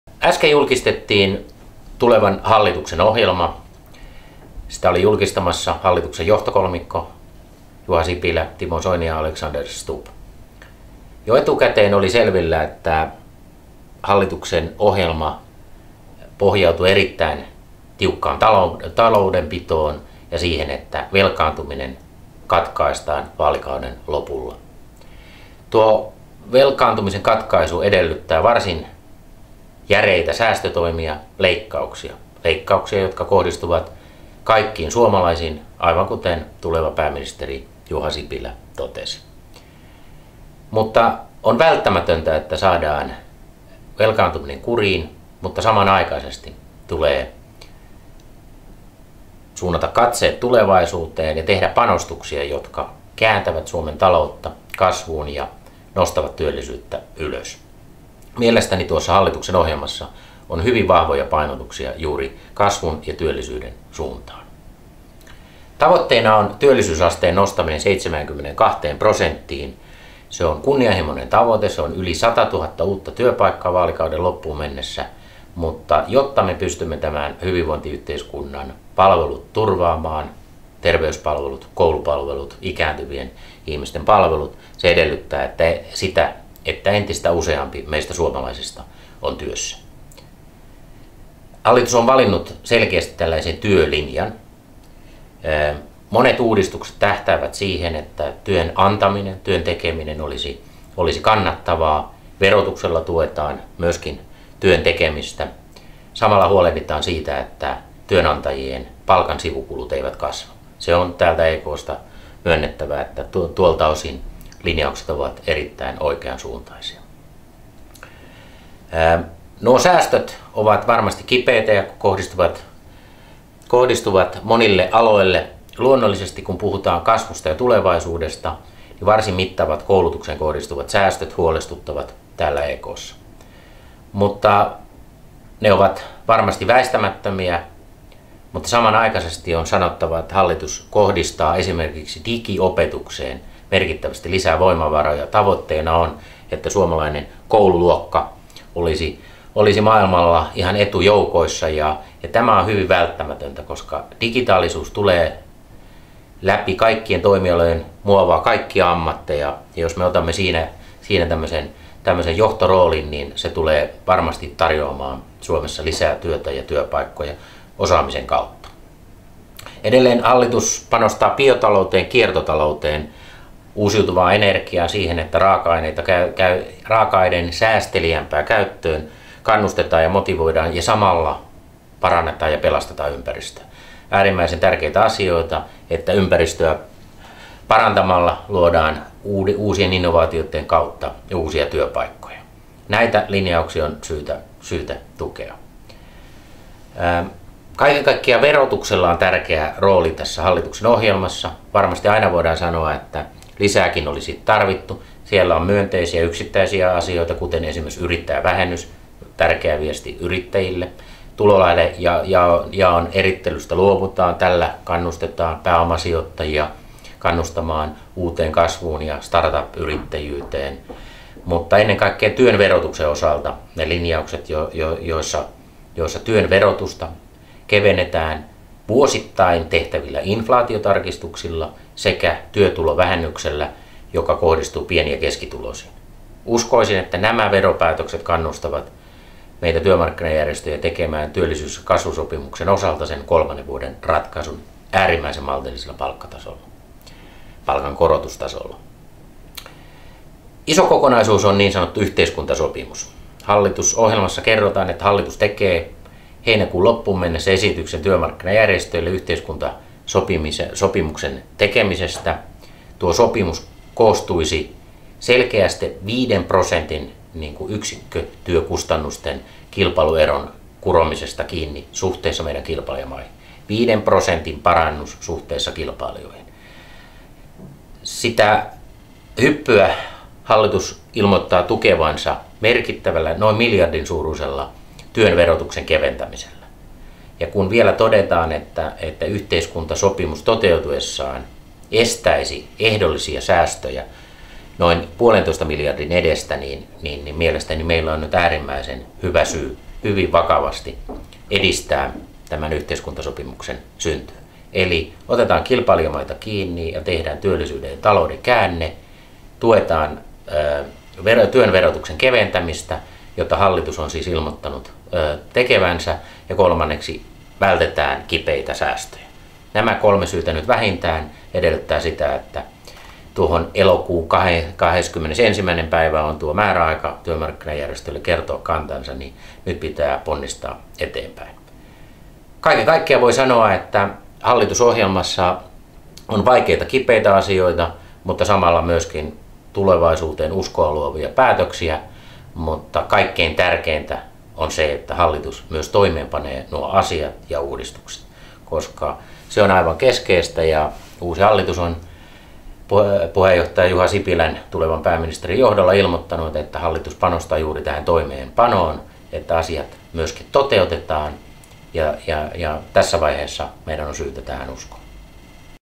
Häkämies kommentoi Sipilän hallitusohjelmaa (6 min)
EK:n toimitusjohtajan Jyri Häkämiehen kommentit tuoreesta hallitusohjelmasta.